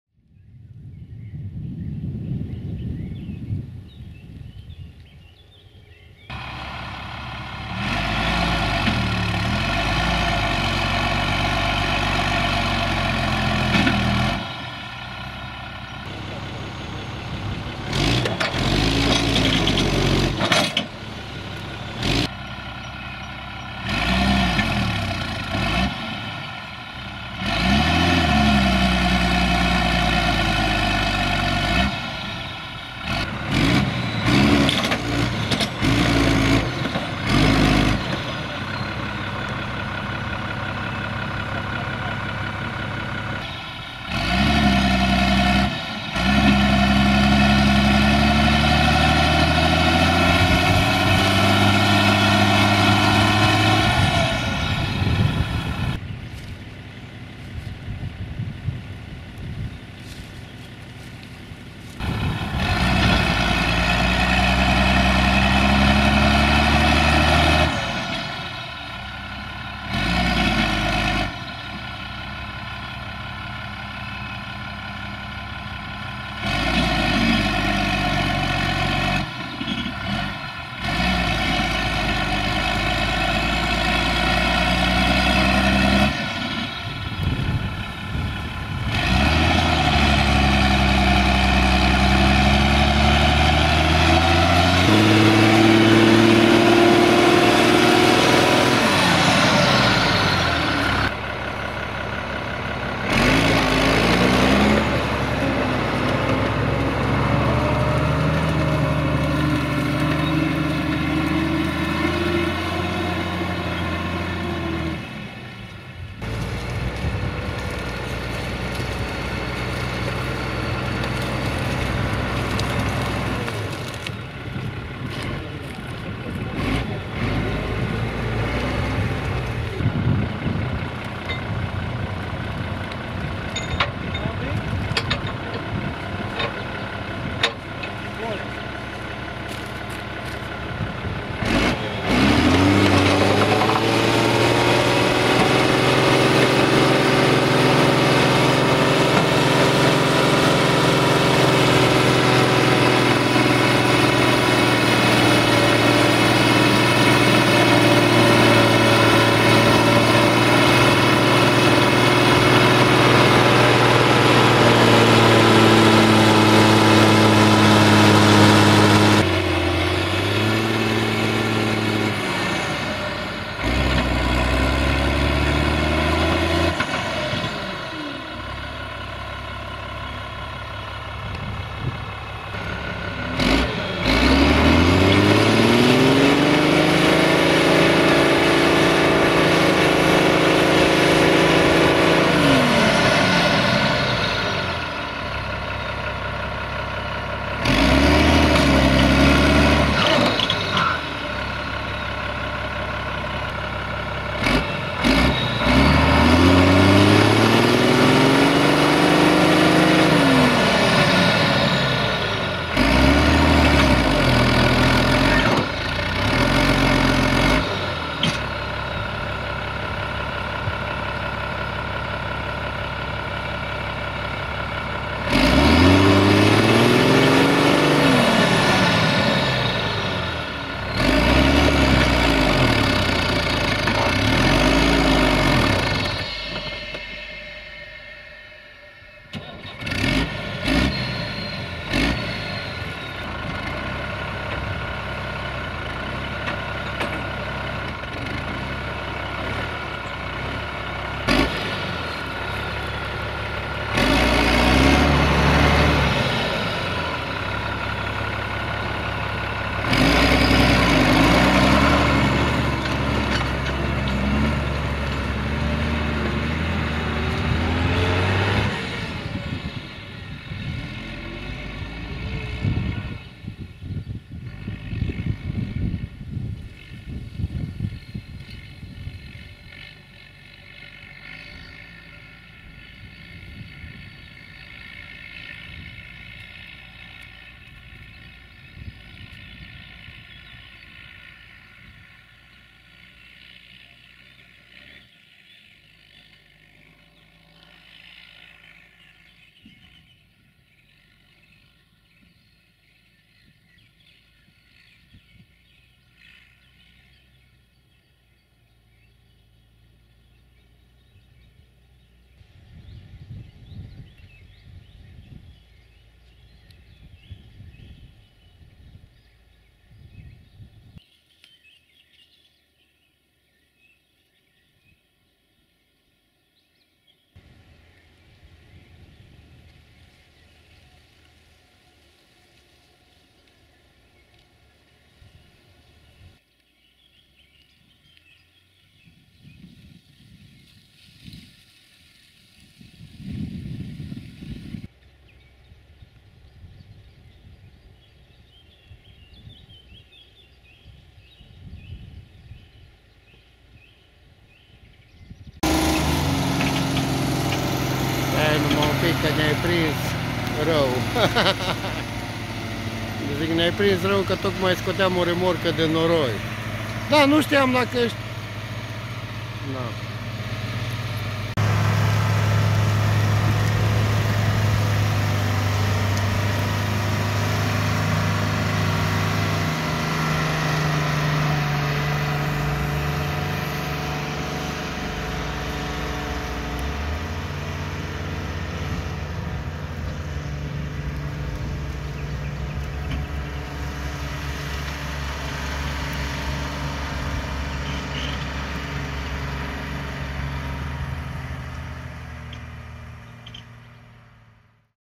🚜 FIAT 1380DT Tractor Stuck with Corn Trailer/Blocat cu Remorca de Porumb - 06 May 2021
00:08 Tractor FIAT 1380DT Tractor & Remorca Încărcată cu Porumb Blocată/Stuck Loaded Corn Trailer
01:42 Manevră/Maneuver Tractor FIAT 1380DT Tractor
02:22 Încercarea a Doua/Te Second Try
04:15 Tractor FIAT 1380DT Tractor Plecă după Ajutor/Go for Help
05:56 Tractor FIAT 1380DT & New Holland Tractor

06 May 2021